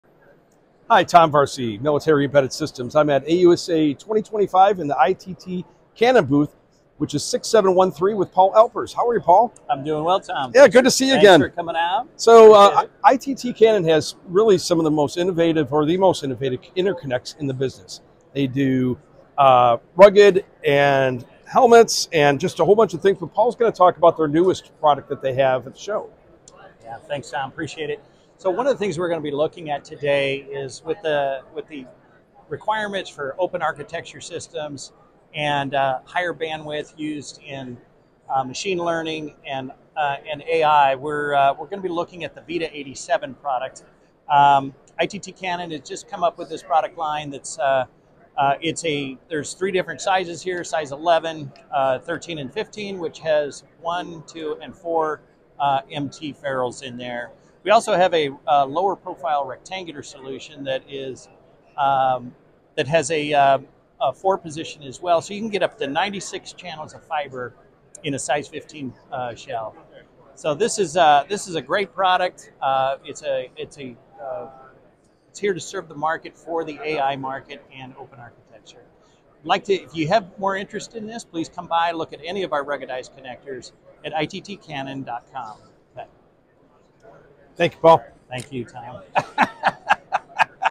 ITT Cannon debuts interconnects at AUSA 2025 for the AI, machine learning market
Paul Alpers, Business Development Manager at ITT Cannon, talks with Tom Varcie of Military Embedded Systems at AUSA 2025 about the new VITA 87 interconnect product serving the military AI and Machine Learning market.